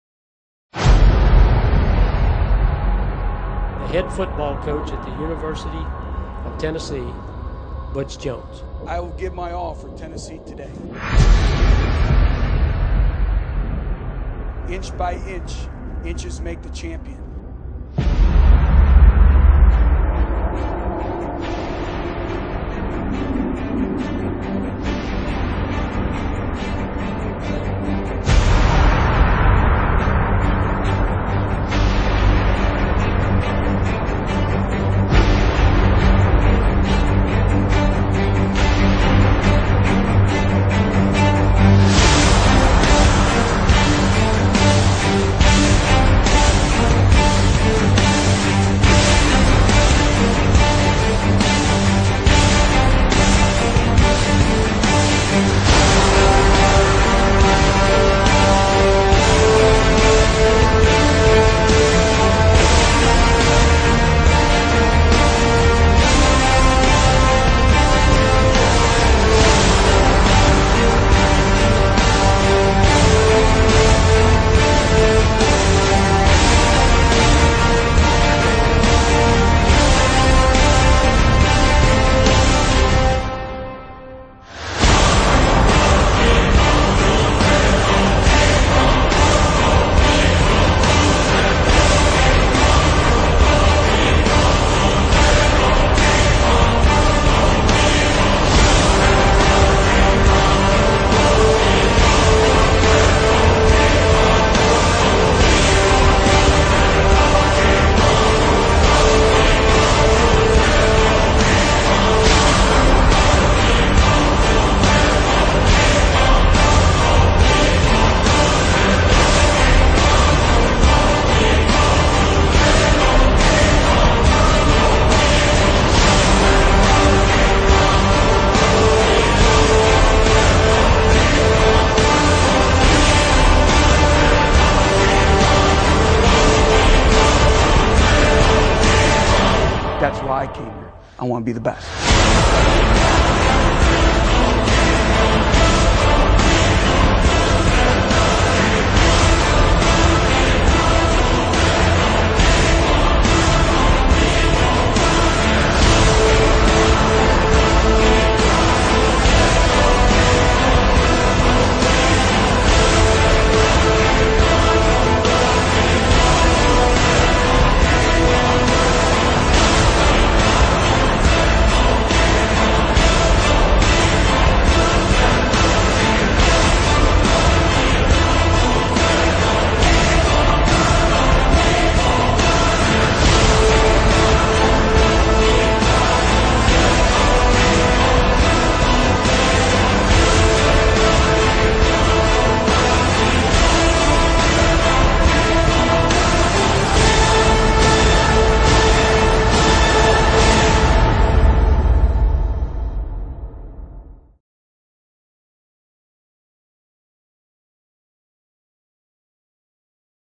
Tennessee Football 2013 Season Preview - Rise To The Top
Preview for the upcoming 2013 football season for the Tennessee Volunteers
All highlights consist of current players